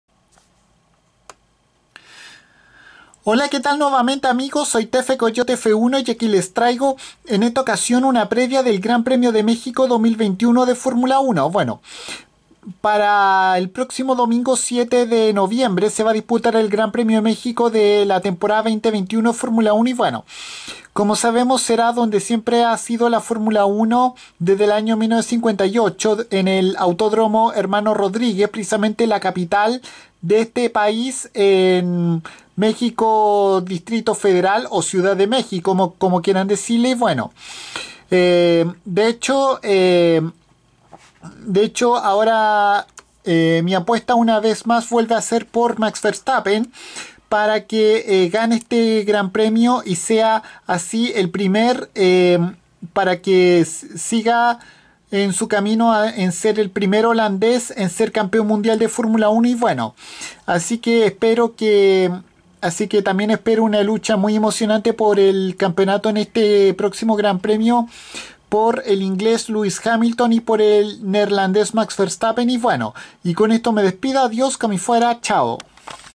Previa GP de México 2021 de F1
Una breve previa de lo que será el Gran Premio de México de la Temporada 2021 de Fórmula 1 que será en el Autódromo Hermanos Rodríguez en Ciudad de México o México D.F